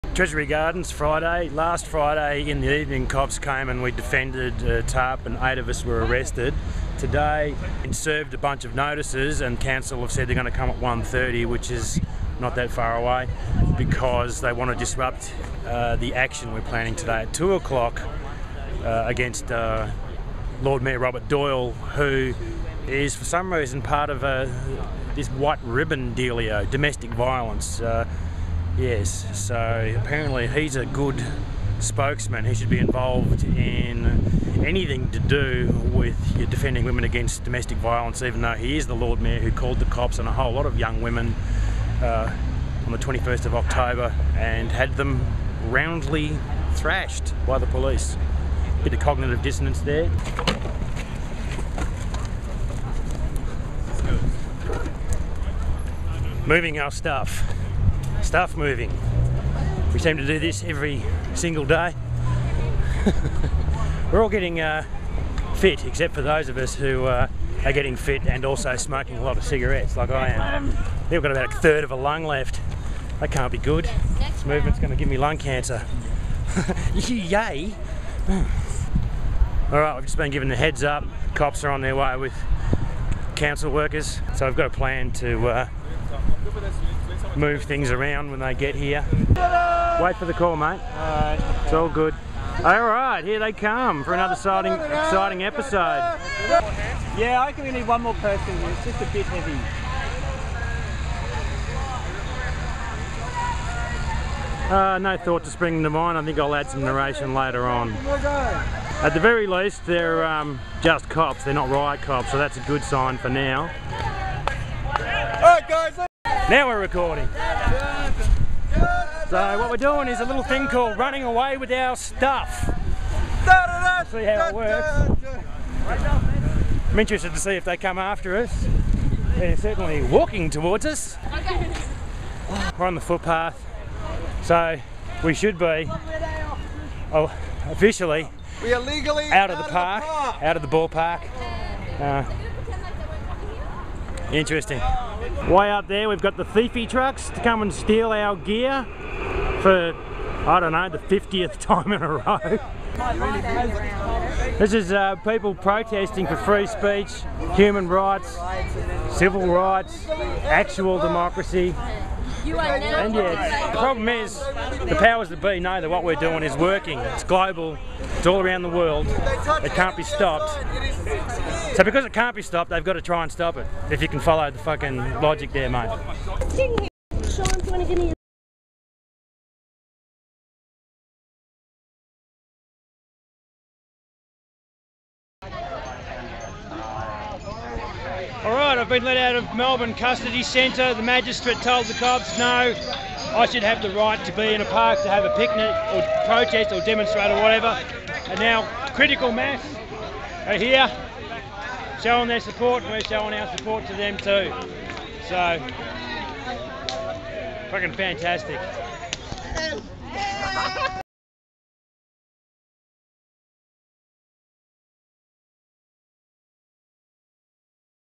Occupy Melbourne: Getting Arrested
Yesterday the Magistrate decided locking me up for the weekend because I might attend a rally in a park is ludicrous.